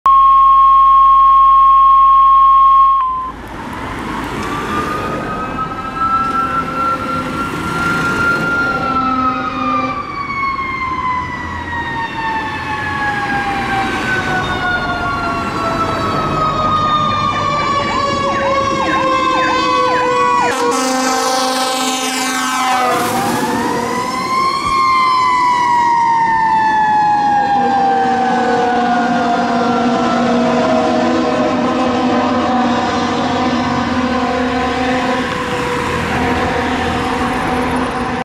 Q10 bomberos ñuñoa rumbo a incendio en Puente Alto
Q10 CBÑ A 10-12 POR 2DA ALARMA DE INCENDIO EN LA COMUNA DE PUENTE ALTO.